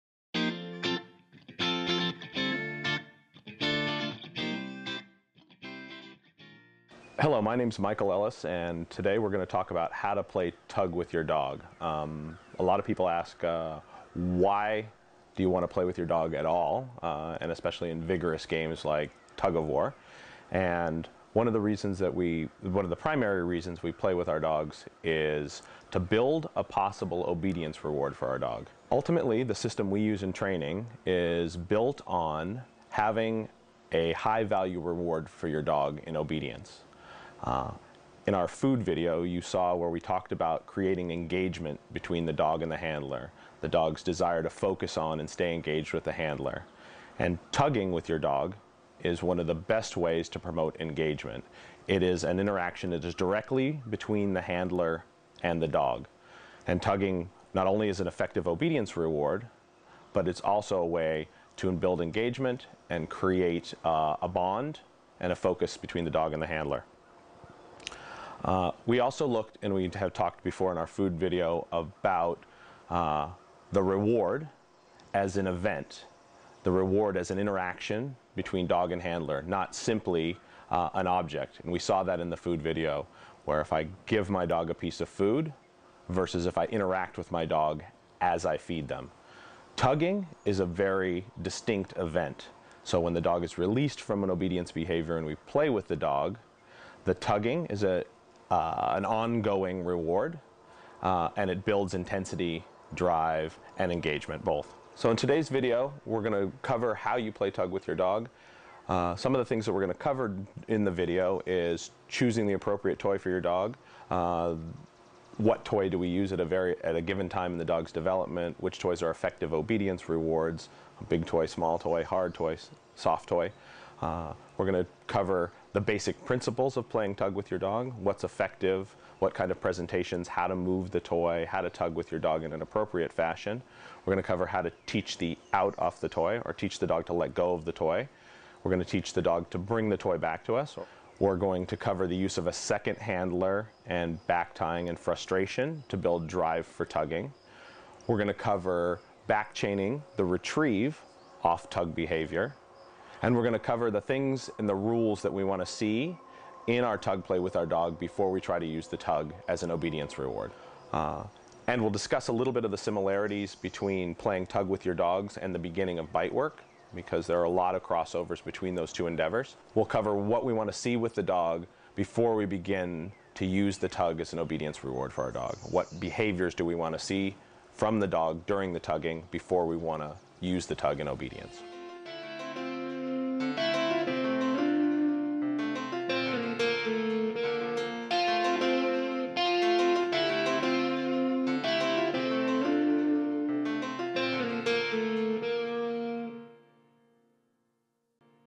Why You Should Teach Your Dog to Play Tug with Michael Ellis
The Power of Playing Tug with Your Dog with Michael Ellis